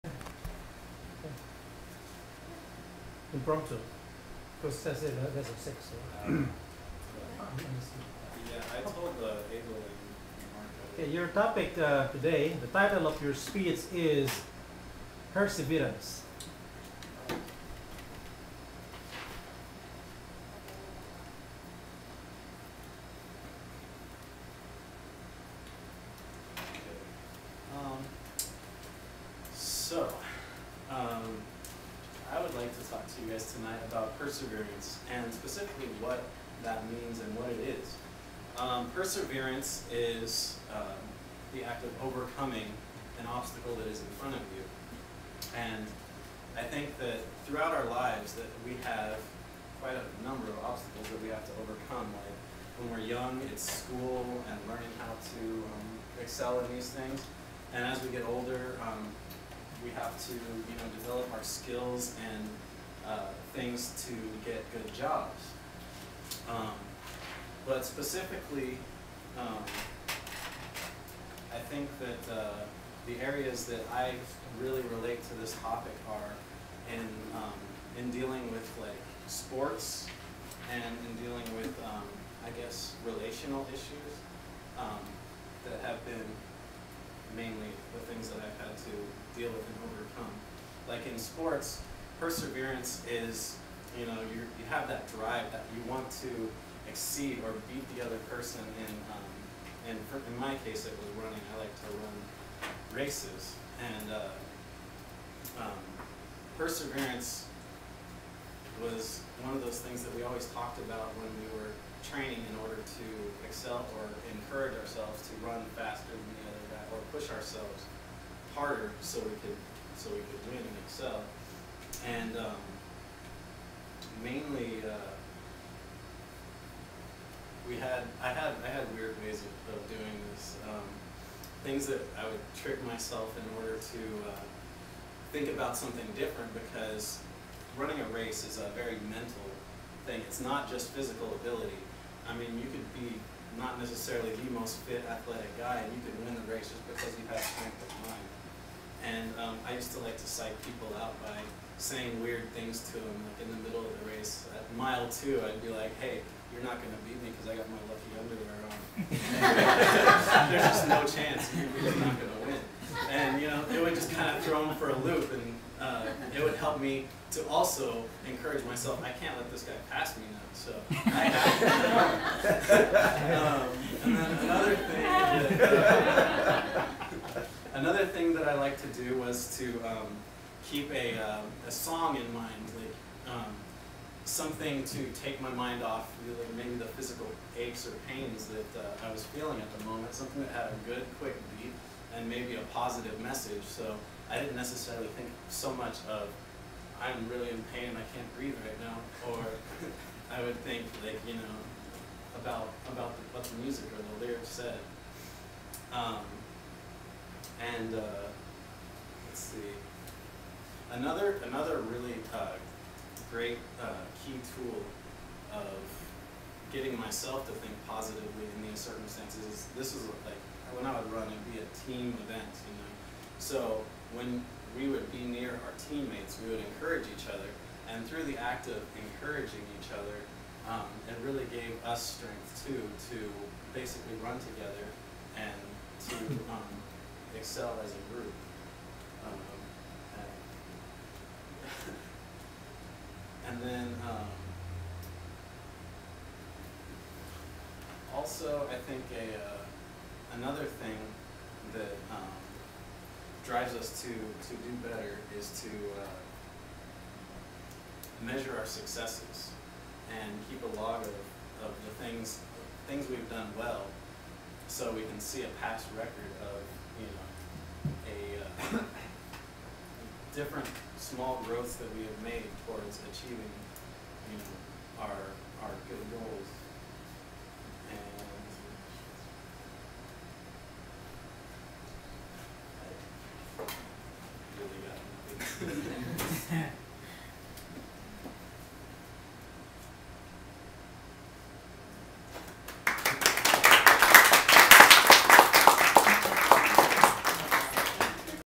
June 27, 2009 Larry O'Bryan Lesson 11 Impromptu speech
Larry O'Bryan "Perseverance"